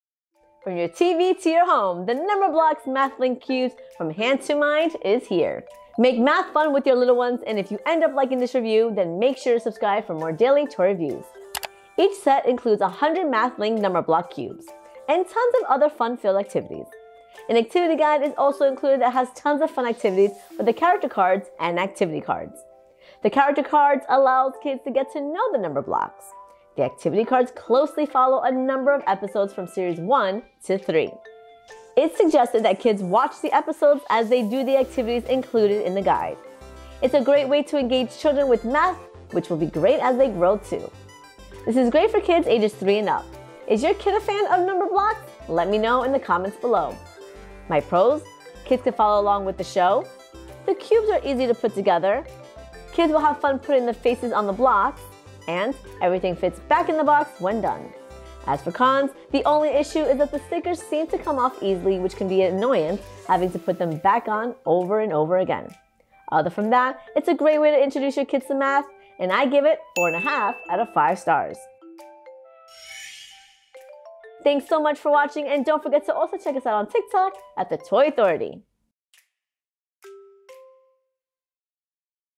Number Blocks MathLink Cubes 1-10 Activity Set
From your TV to your home, the Number Blocks MathLink Cubes from hand2mind is here! For full review and shopping info

Fans of Numberblocks from the hit TV series brings math learning to life. Kids will see how numbers really work as they build their own Numberblocks!